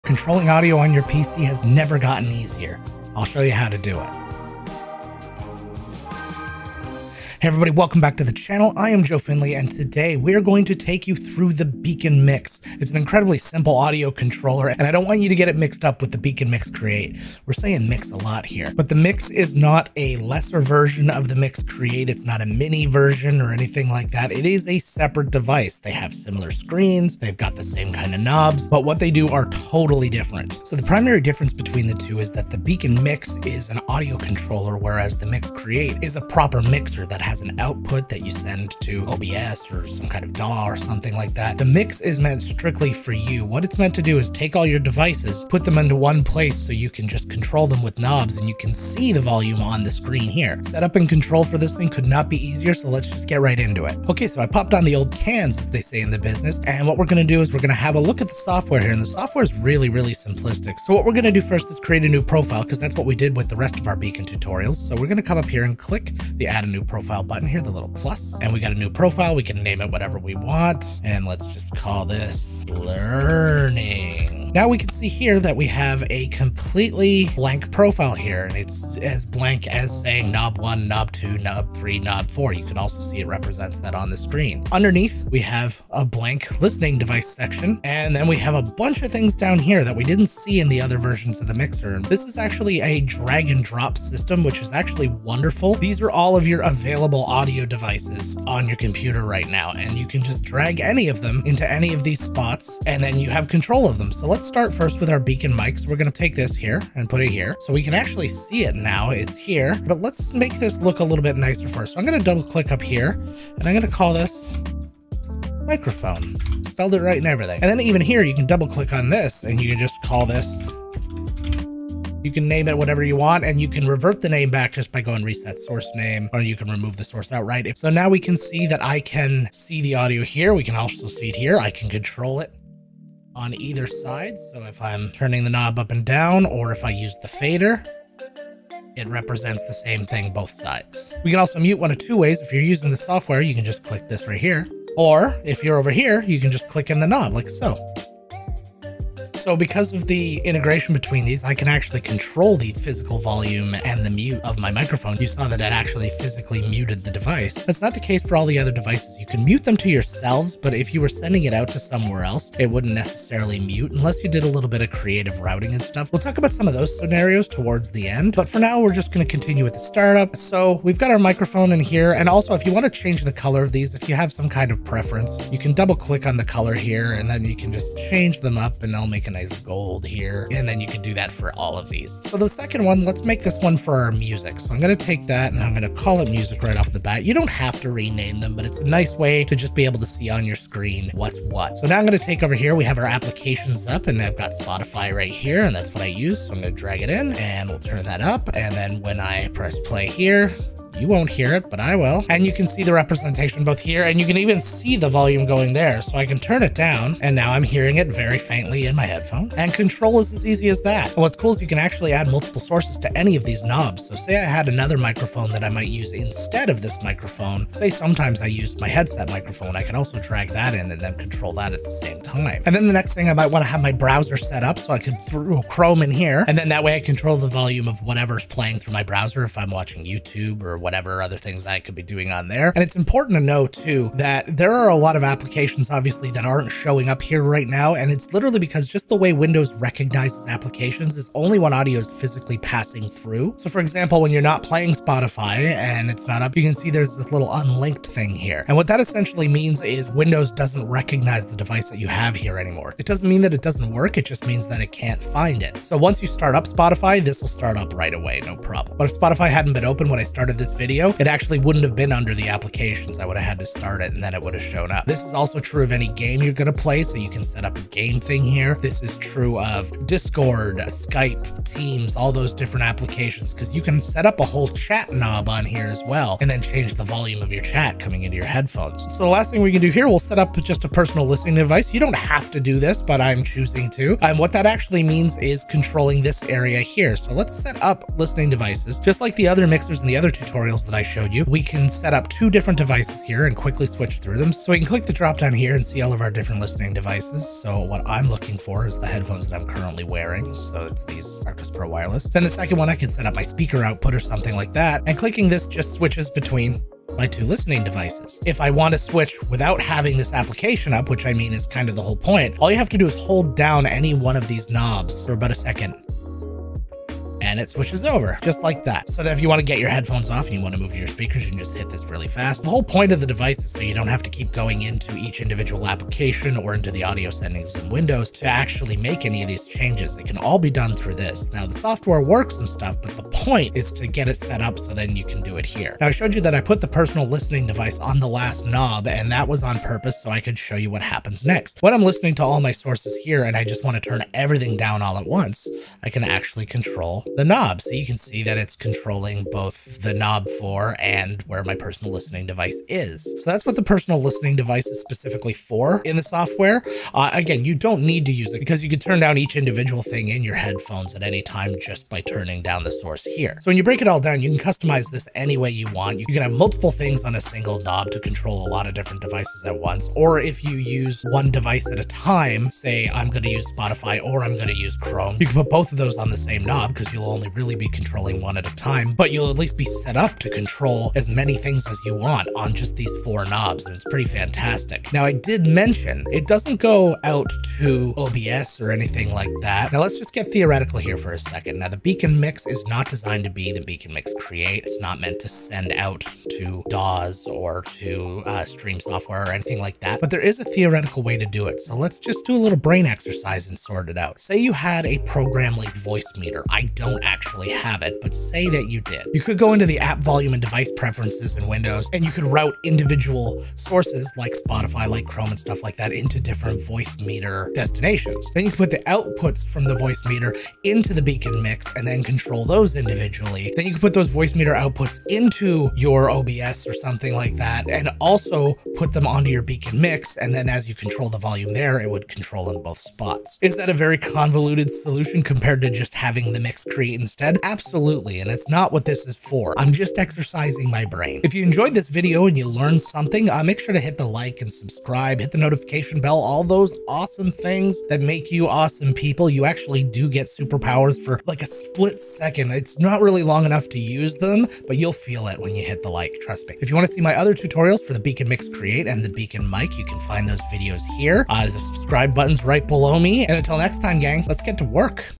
A Complete BEACN Mix Tutorial in 7 MINUTES!
In our final tutorial of the BEACN Series, we learn how to use the BEACN Mix, from setting up a profile and your sources, including putting more than one source on a single knob to customizing the labelling of your knobs.

I also conduct a little thought excercise where I make a BEACN Mix act a little bit more like a Mix Create (though not exactly the same).

Our Sub Goal! 398% 2.99K/750

Welcome to Our Latest Discord Member ➡ mashpotatohater♯0

📖Chapters📖
0:00 - Intro
0:31 - The Difference Between The Mix and Mix Create
0:54 - BEACN Mix Software
1:01 - Create a New Profile
1:28 - Drag and Drop Sources
1:50 - Customize Your Knobs
2:21 - Muting
3:31 - Multiple Sources Per Knob
3:52 - Why Can't I Find My Source?
4:39 - Control Your Personal Listening Device
6:25 - A Thought Excercise

All support goes toward making more and better videos.